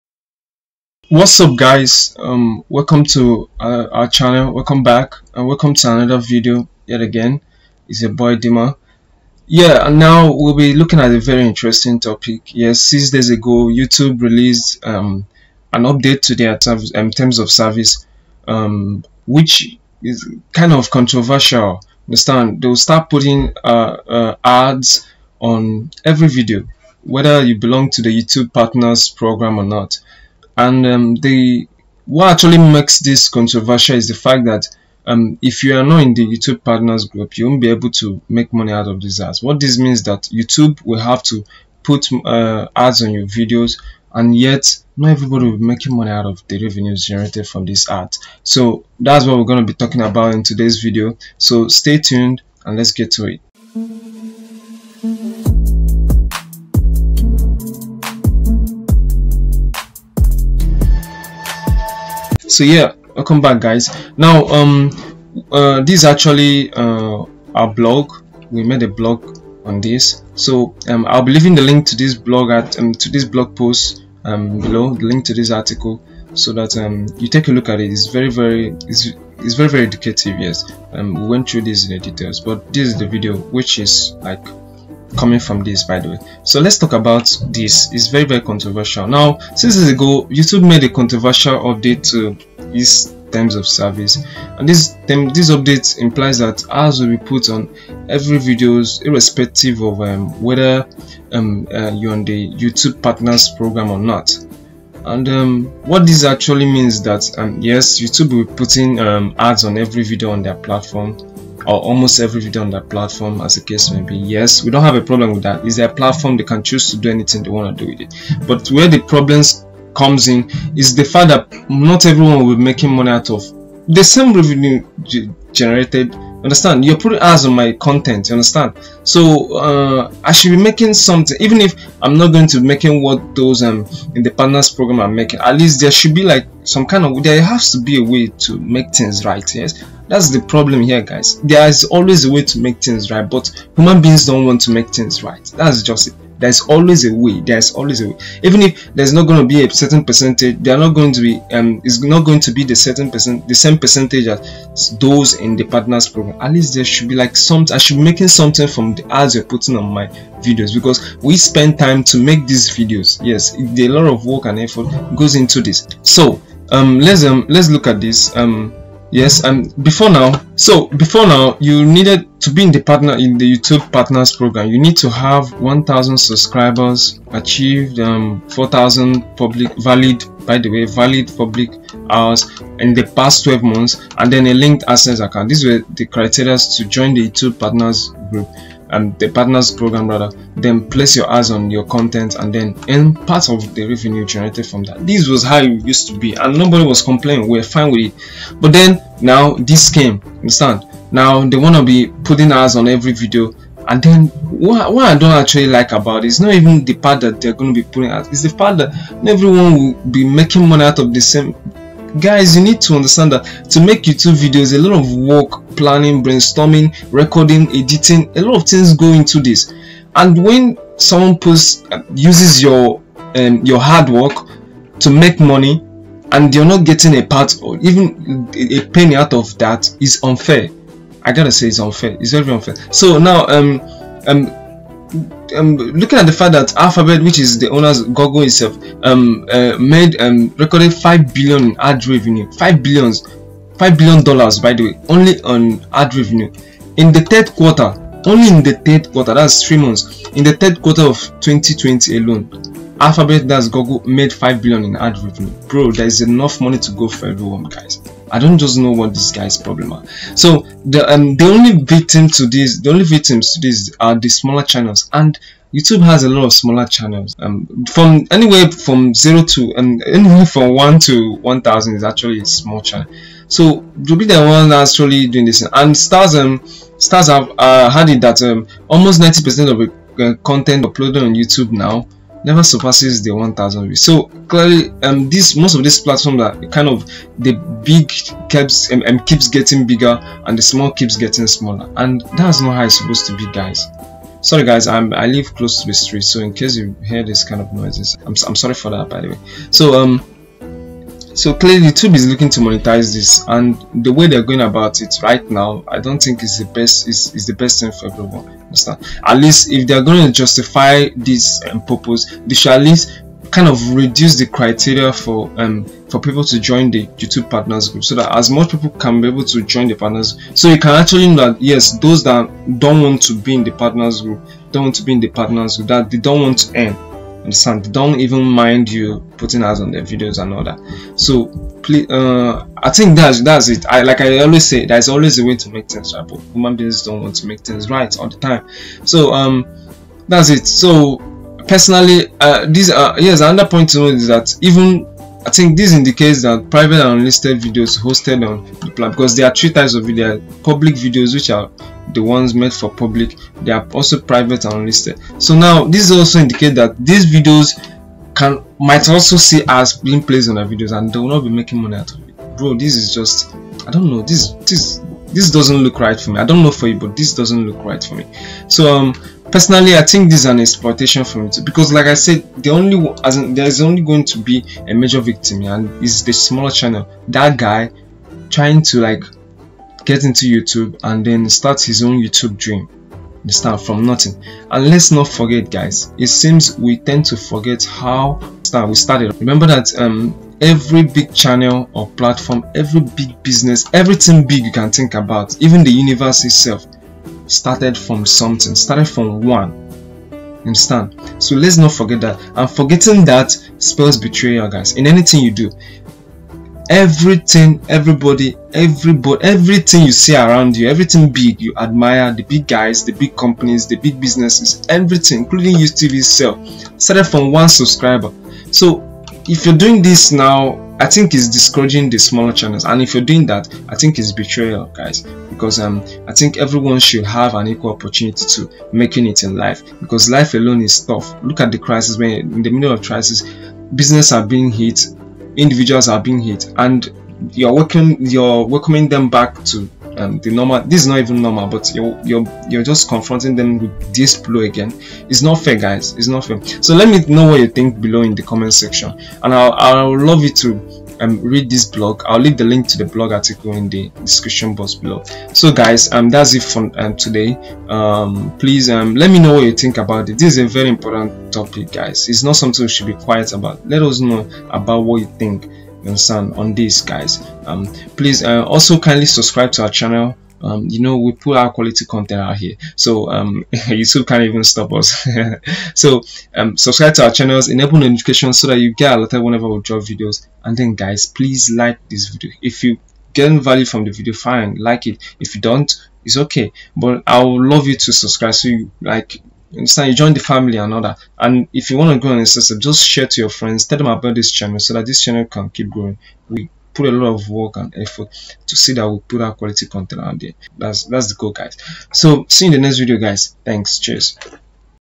Youtube Monetization Update: YouTube won't pay SMALL CHANNELS for Ads!!!
This update implies that YouTube will be putting ads on non-monetized channels and due to these channels not being in the Youtube’s Partners Program, they won’t be making any share of the revenue generated form this!

Wanna know the full details of this and the effects? Watch till the end and find out!

Also drop a "Like" and "Share" our video so it gets recommended to others, to get value from. Ask any questions and get interactive in the comment section; we are always there!

Check us out today on our website to learn more about what we do: